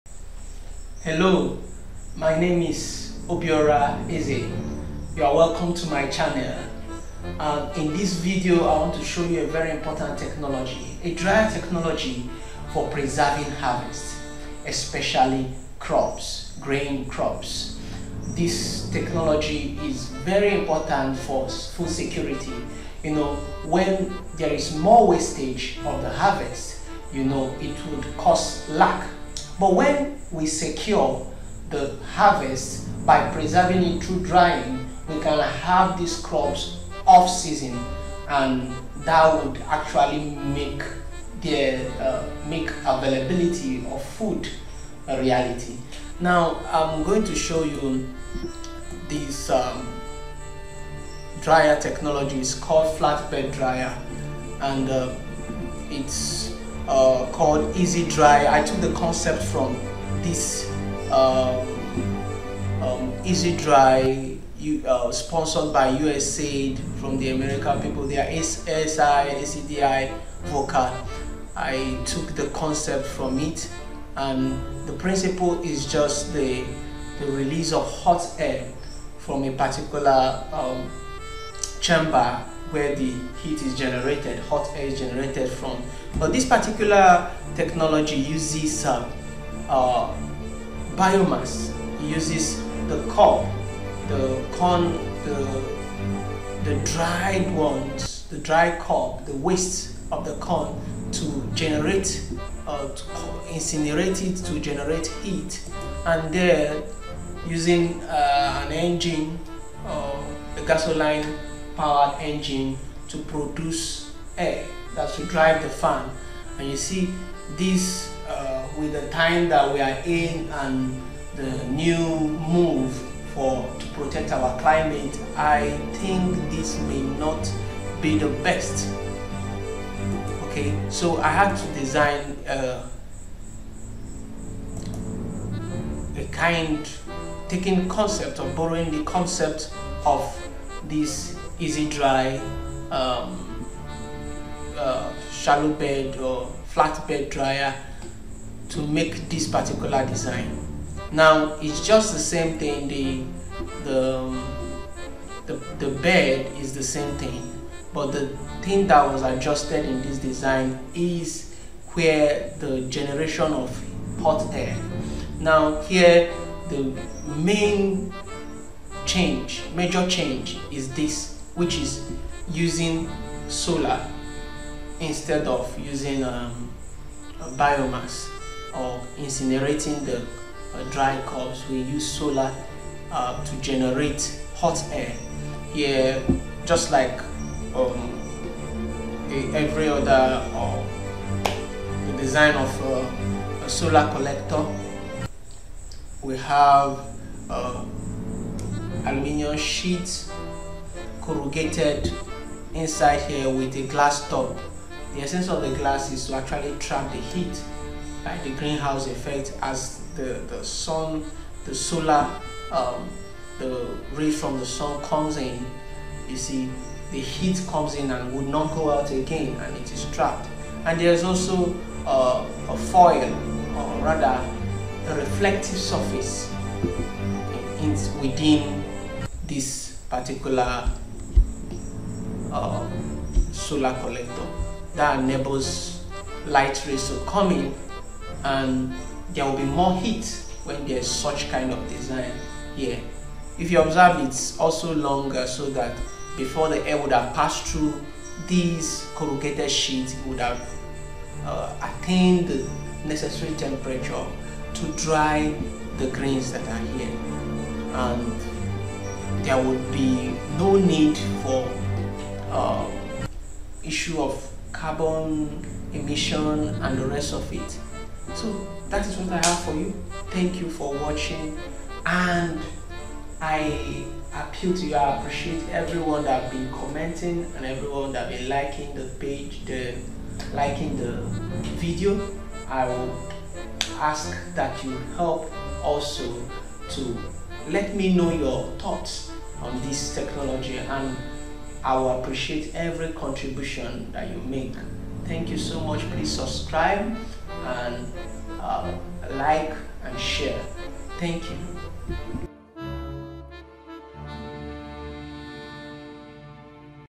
Flat Bed Solar Dryer
This video uses the concept of EasyDry M500 Flat Bed Dryer to design a solar Flat Bed Dryer for grain drying and preservation.